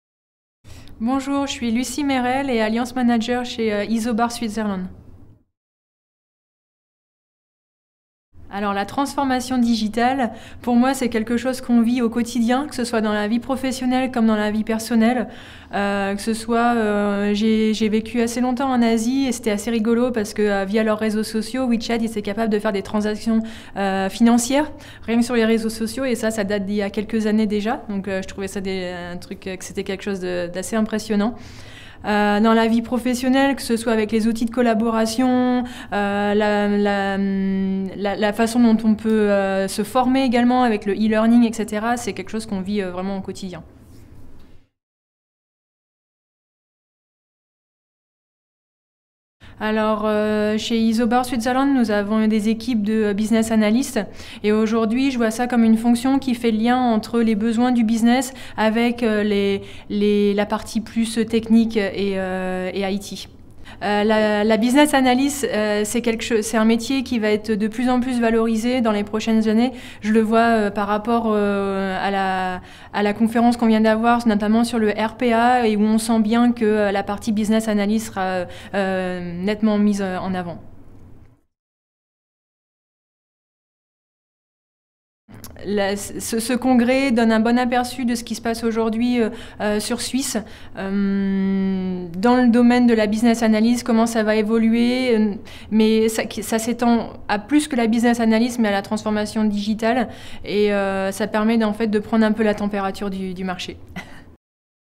Congrès de la Business Analyse 2019 - Interview avec le Business Analyste Lucie Mérel
- IIBA Geneva, chapitre suisse romand de l'IIBA
- Congrès annuel de Bussiness Analyse organisé par IIBA Geneva